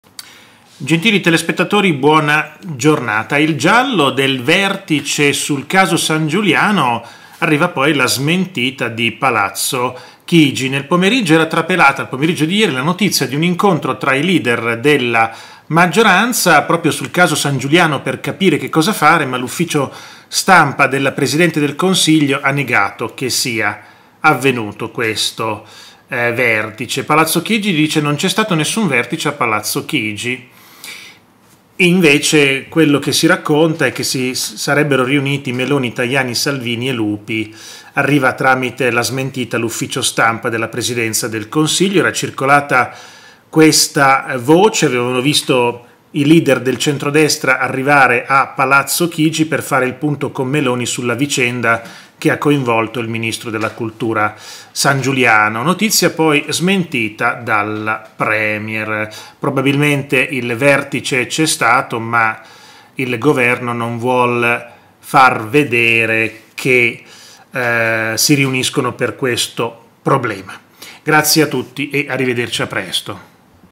VERTICE DELLA MAGGIORANZA SUL CASO SANGIULIANO. MA LA PRESIDENZA DEL CONSIGLIO DICE CHE..
► Canale di Luca e Karla Vincitore

► I miei libri: